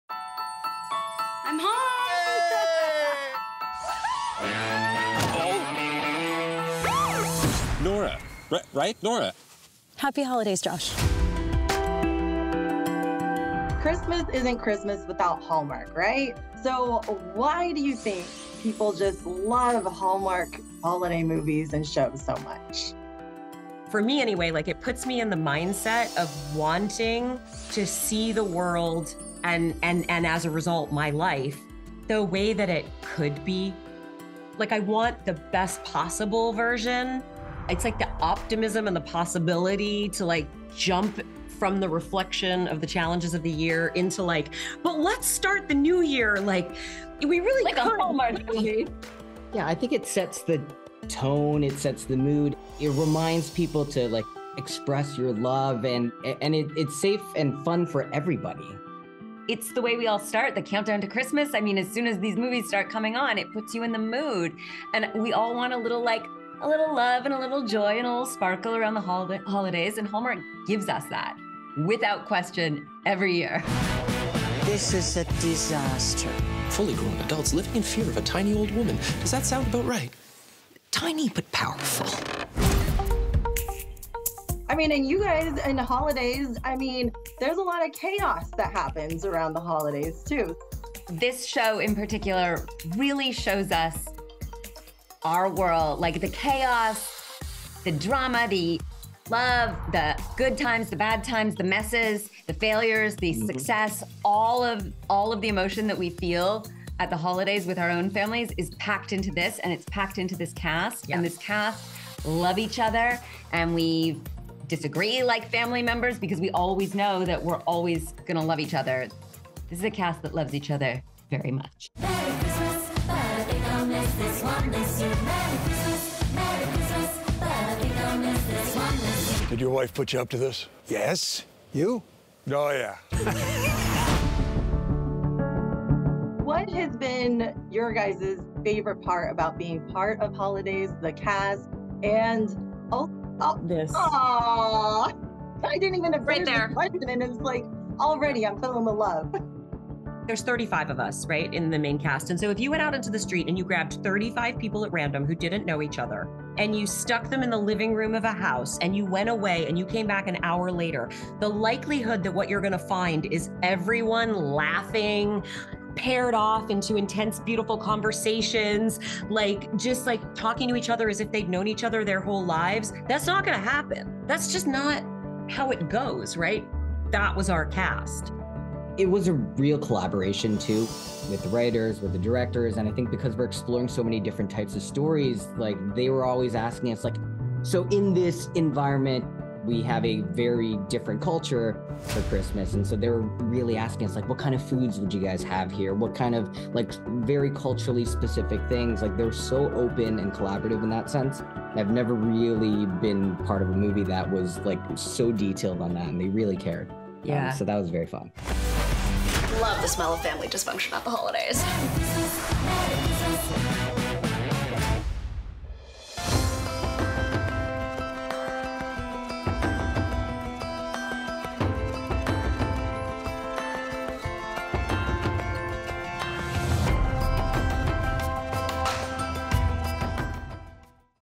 The Cast of "Holidazed" interview
Talented trio Lindy Booth, Osric Chau, and Rachelle Lefevre tell host Liana Brackett about working together in the Hallmark original series "Holidazed" and why they were impressed with the massive ensemble cast.